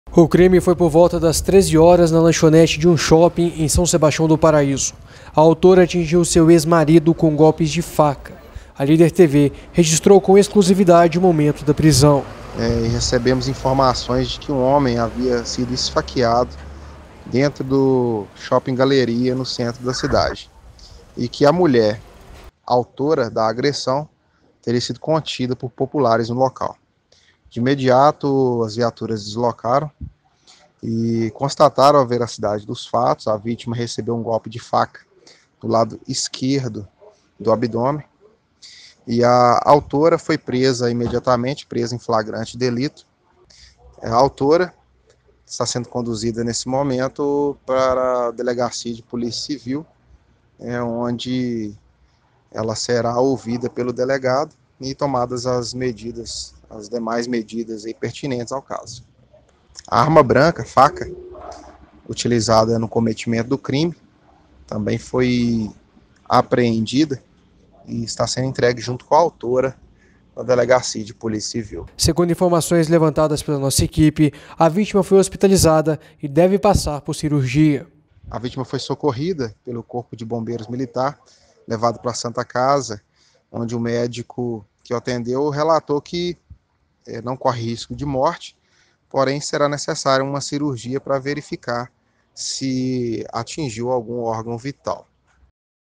Mulher esfaqueia ex-marido em shopping de São Sebastião do Paraíso
Um crime passional paralisou o Centro de São Sebastião do Paraíso na tarde desta quarta-feira (27).